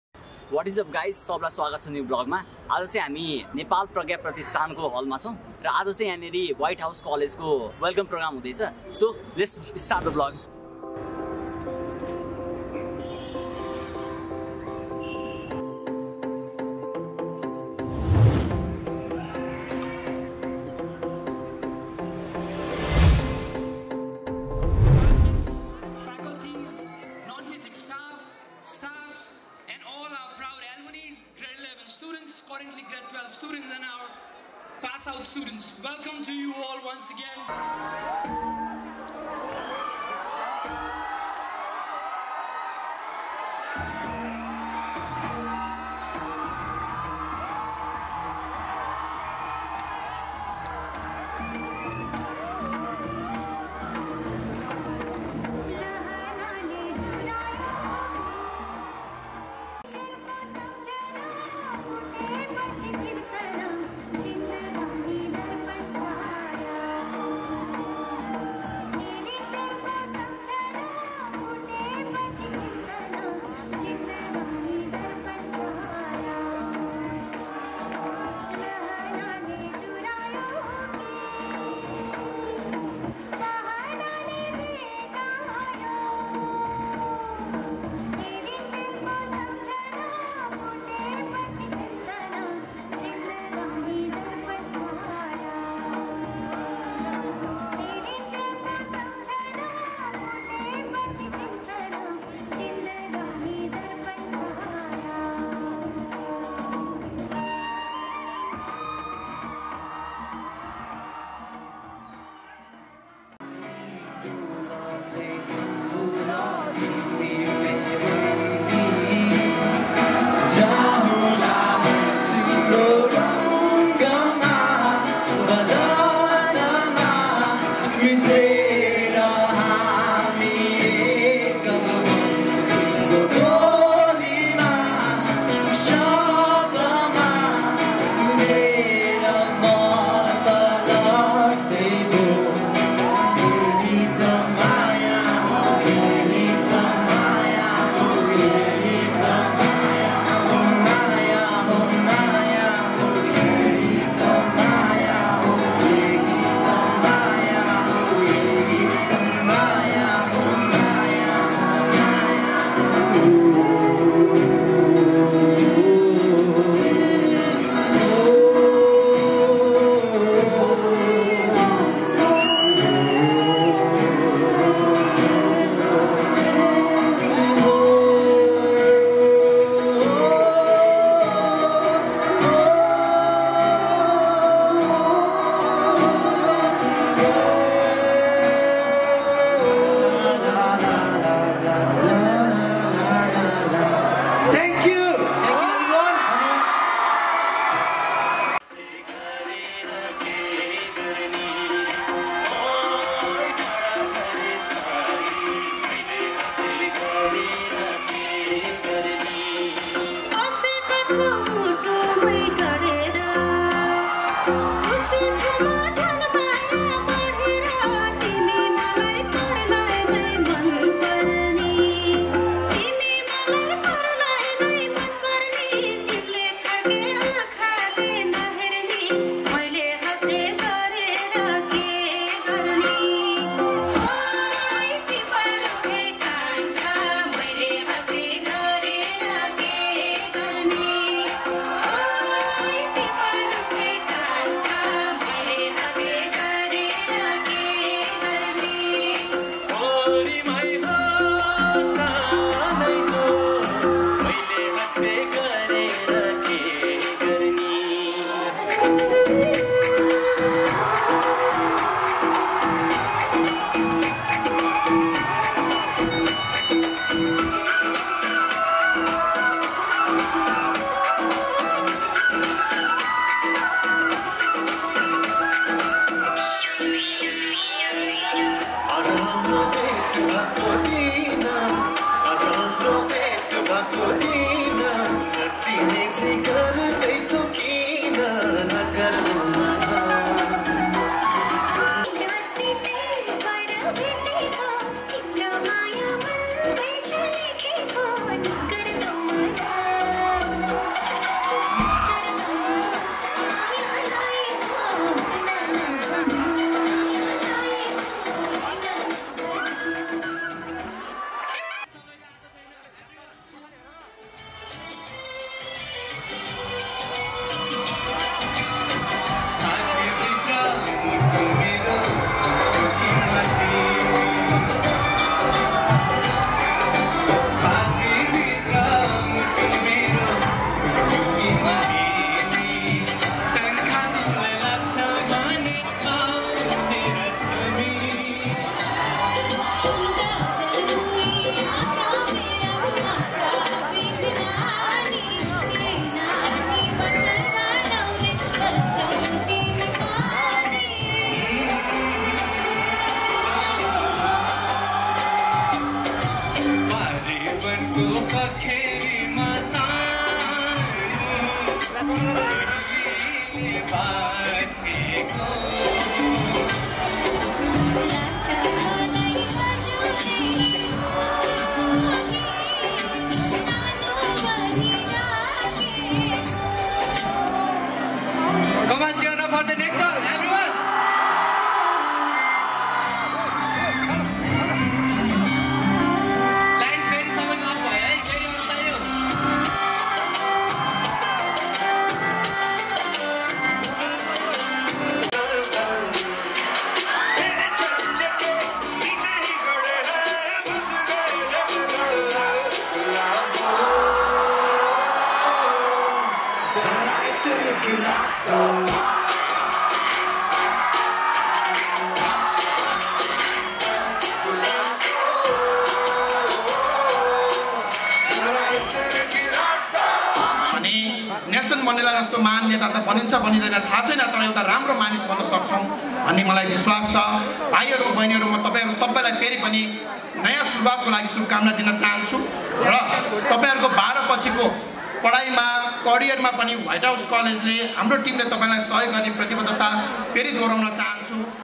Welcome program Himalayan Whitehouse College Tinkune Kathmandu || Nepal Academy Hall || DDTwinsVlog
Welcome program in Himalayan whitehouse college, Tinkune.